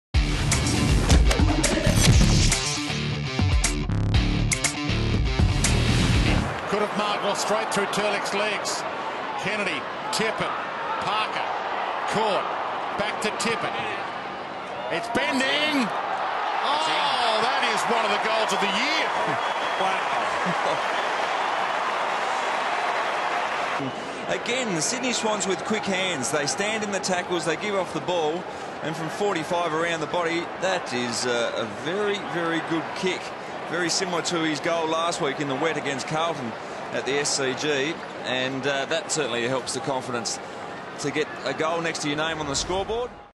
Kurt Tippett drill another potential goal of the year nomination R15 2013
Swans new recruit Kurt Tippett has shown some clean skills in his first few rounds in the red and white